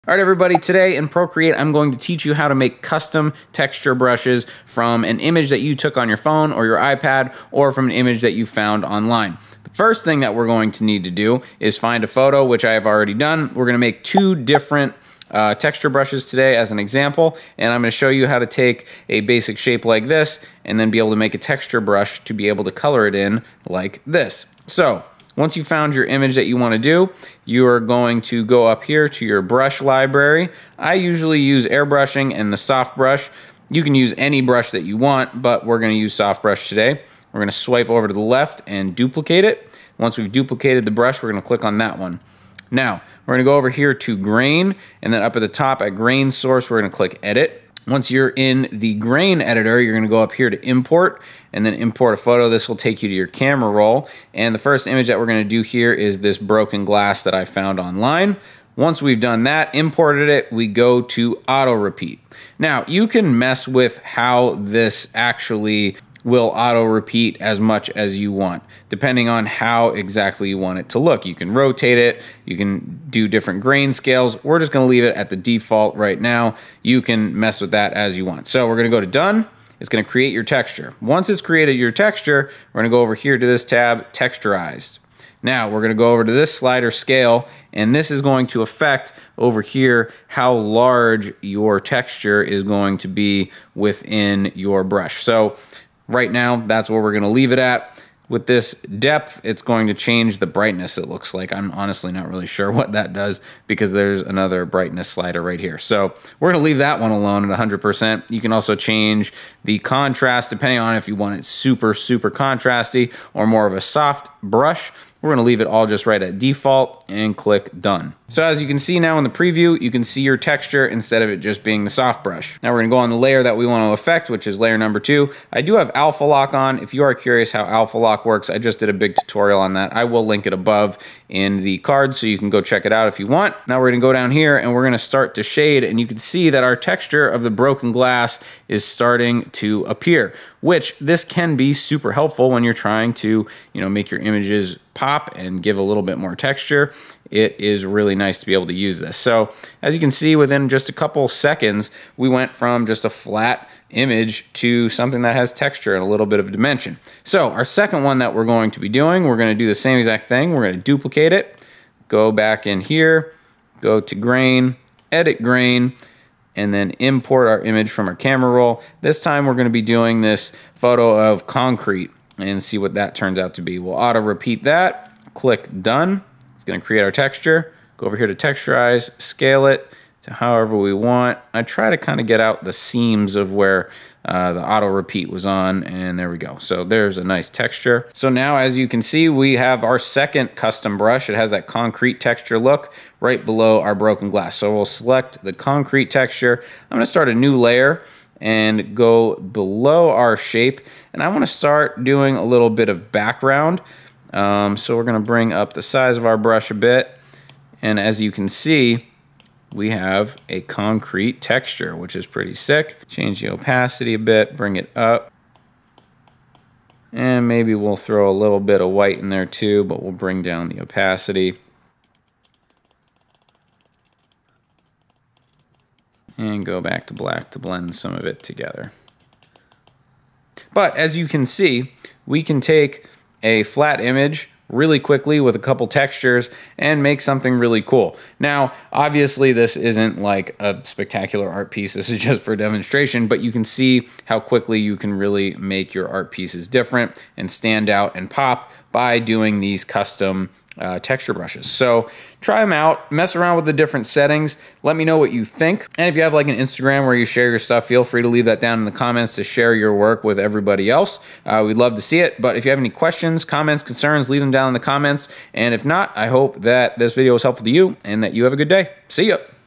How To Create A Custom Texture Brush From A Photo In Procreate
In this Procreate tutorial I will show you How To Create A Custom Texture Brush From A Photo In Procreate.  This is super helpful in putting together an art piece, but also its fun to go out to find textures in the real world to be able to make custom brushes in procreate with!